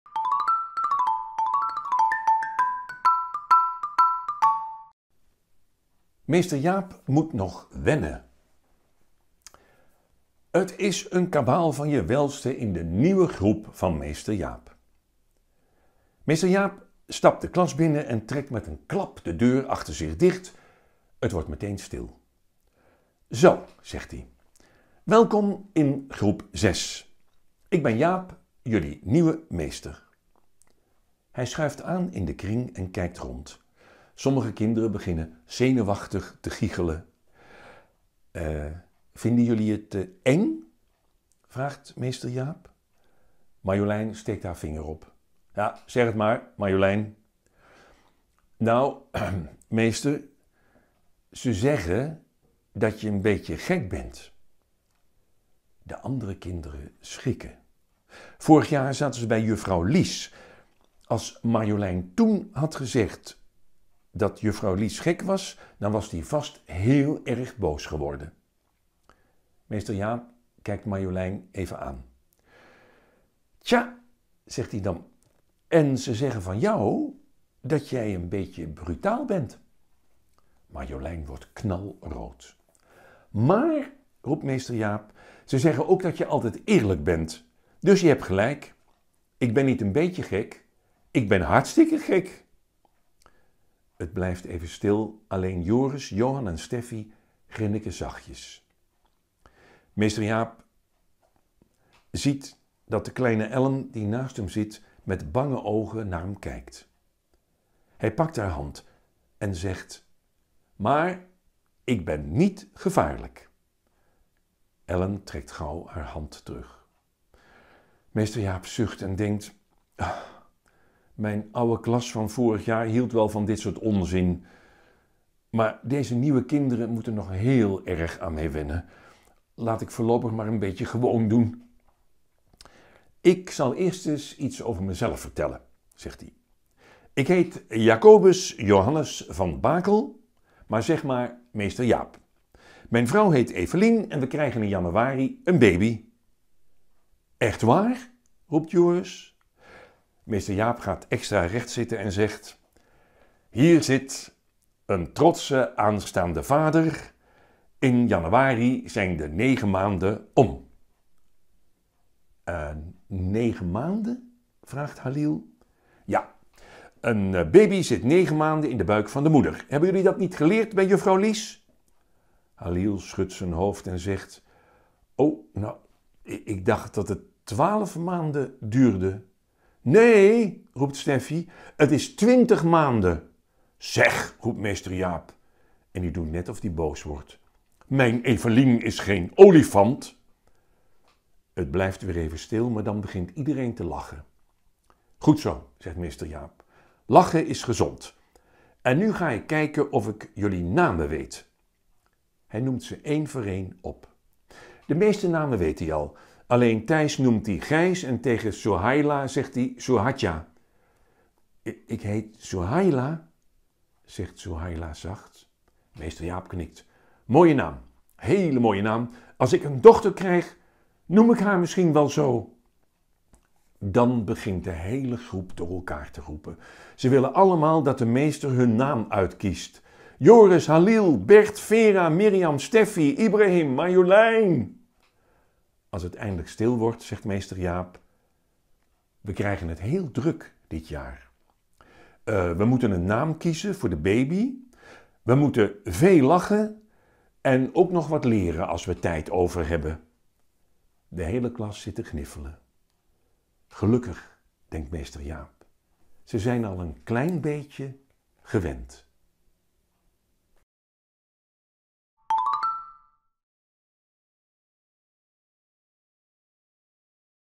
13 Meester Jaap moet nog wennen
Jacques Vriens leest voor uit Meester Jaap